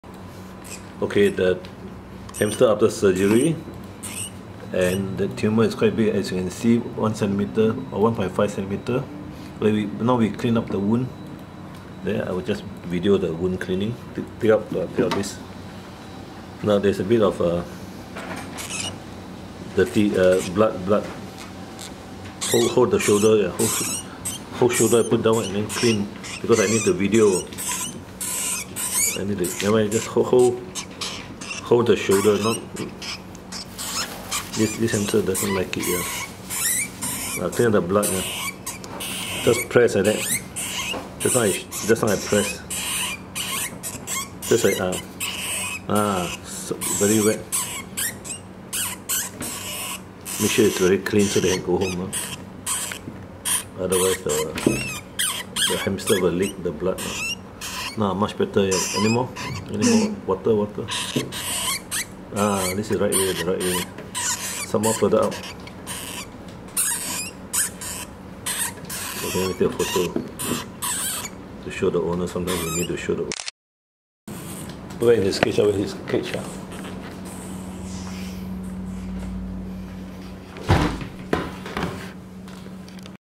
How a large tumour in a 2.5-year-old hamster is excised. Pt 2/2
After surgery, hamster is cleaned properly before going home. Protesting.